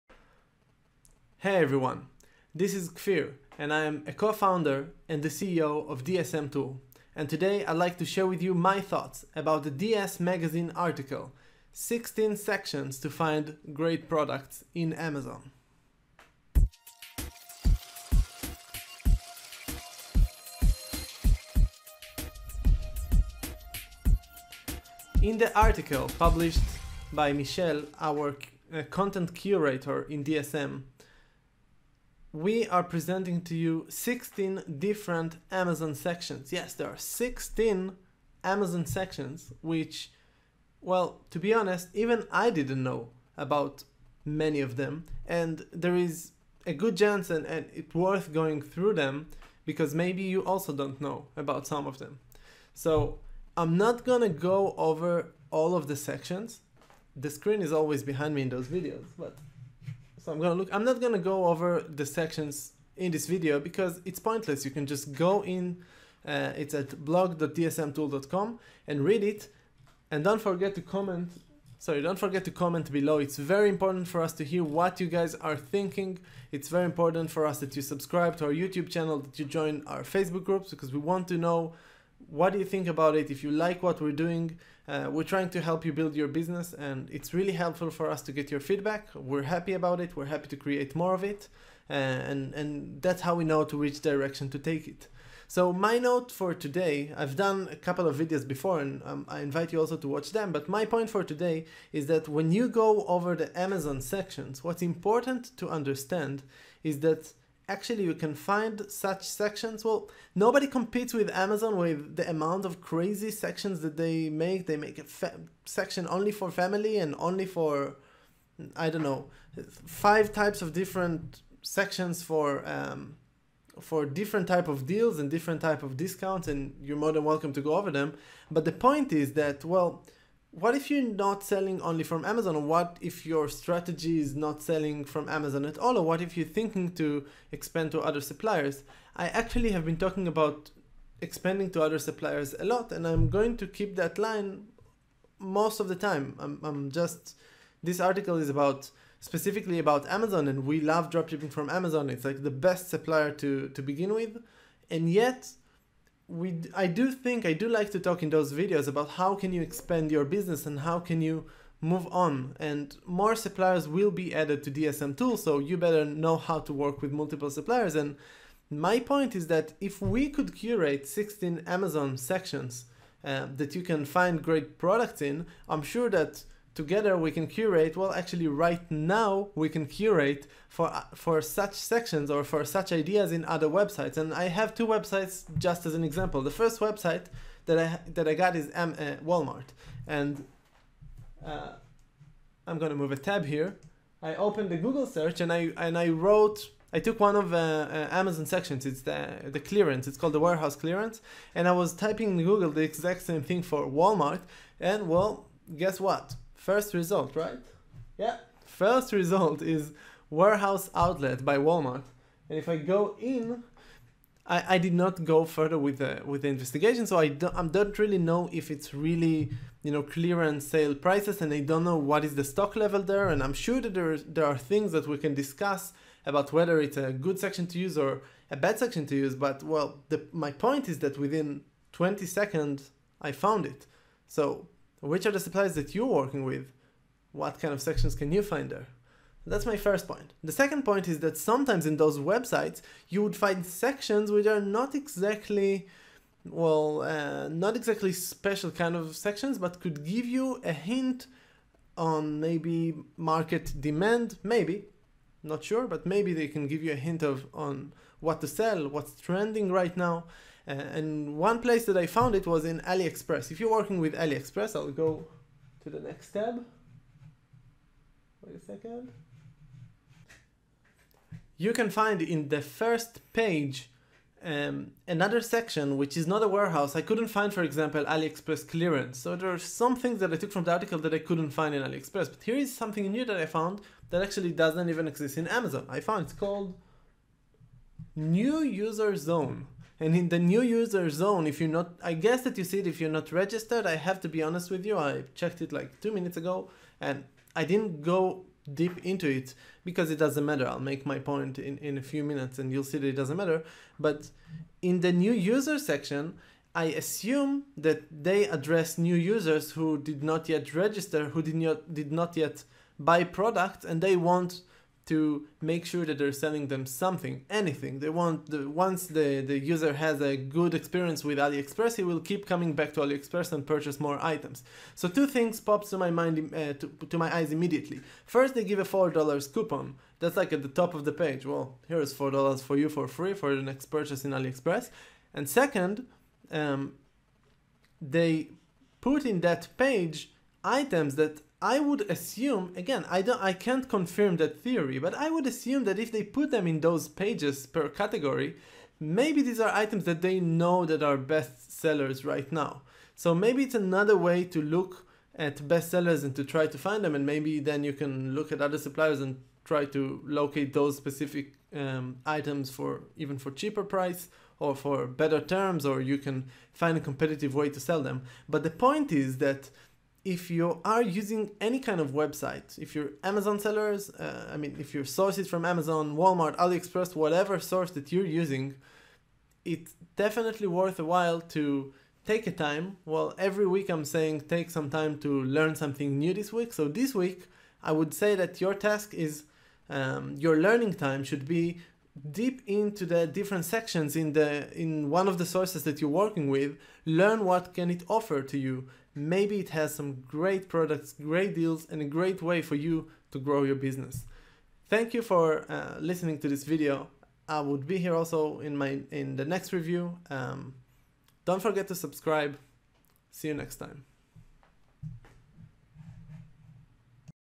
DSMagazing: CEO's Review - 16 Amazon Sections to Find Great Products
Kfir Shapira, our CEO, will talk about 16 Amazon sections to find great products.
Would you like to know what it's all about? Don't miss it!

Learn what you need to know about drop shipping at the Dropship Academy: